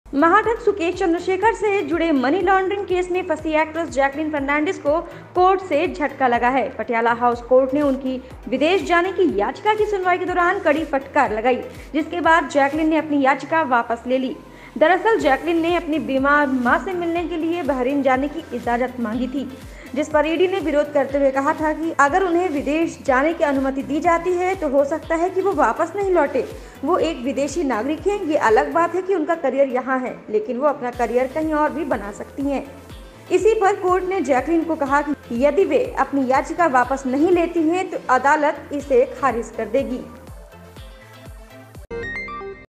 मां से मिलने बहरीन नहीं जा पाएंगी जैकलीन फर्नांडिस
जैकलीन फर्नांडिस को बीमार मां से मिलने के लिए बहरीन जाने की कोर्ट से इजाज़त नहीं मिली.